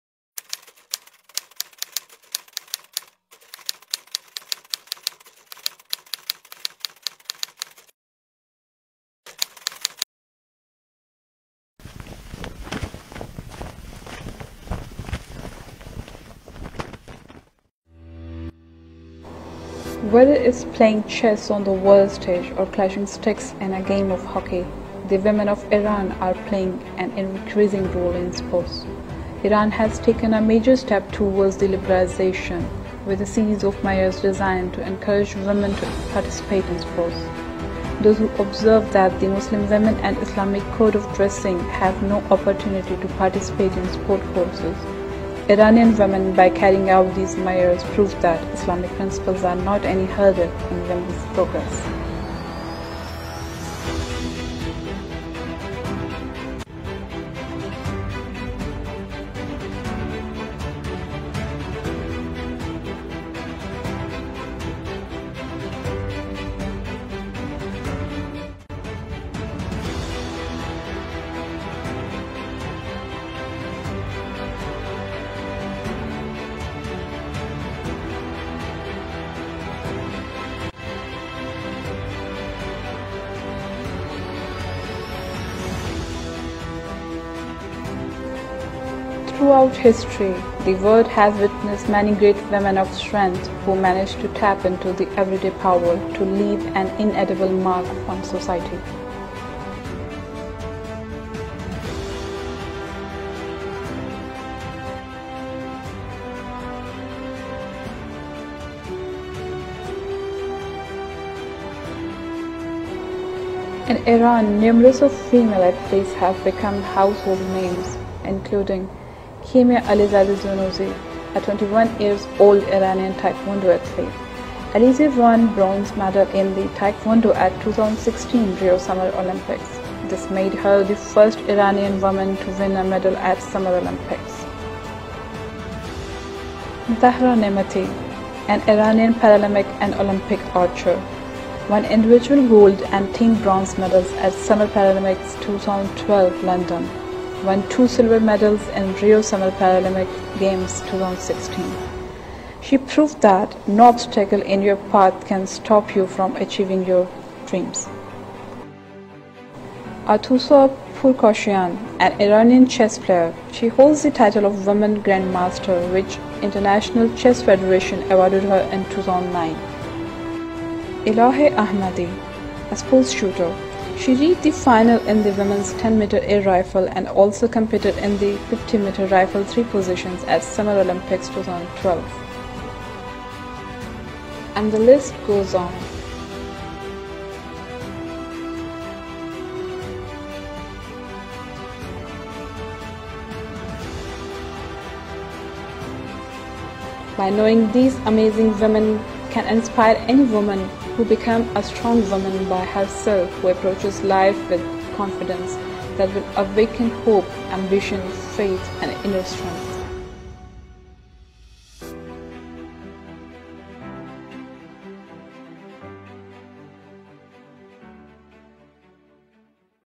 Iranian Women Athletes at International Sports Competitions and Olympics
“Women athletes play a valuable role in promoting Iran’s values abroad and raising national self-confidence at home. They act as ambassadors at international competitions, presenting Iran as a “determined, religious, talented and noble nation, which is committed to Sharia (Islamic law).”
(Supreme Leader Ayatollah Ali Khamenei)